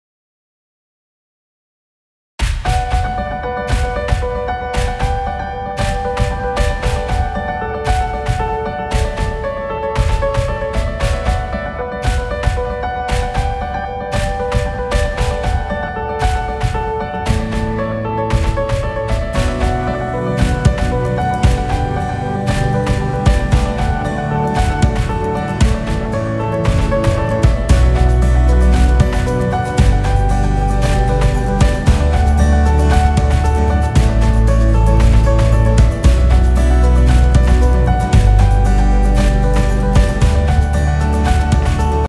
Hindustan में Mosques को नहीं बनाया जाता नि-शाना- Pak Defense Minister का बयान
पाकिस्तान के शहर पेशावर की मस्जिद में ध-मा-का हुआ है। इस घटना के बाद पाकिस्तान के रक्षा मंत्री ख्वाजा आसिफ का बयान वायरल हो रहा है। पाकिस्तान की संसद में उन्होने india और israel को लेकर जो बयान दिया है उसकी खूब चर्चा हो रही है। उन्होने कहा कि, भारतऔर इजरायल में भी मस्जिदों को निशाना नहीं बनाया जाता है। वहां नमाजिदों को मारा नहीं जाता है।

There has been a b' last in the mosque of Peshawar city of Pakistan. After this incident, the statement of Pakistan's Defense Minister Khwaja Asif is going viral. The statement made by him in the Parliament of Pakistan regarding India and Israel is being discussed a lot. He said that mosques are not targeted even in India and Israel. There the worshipers are not killed.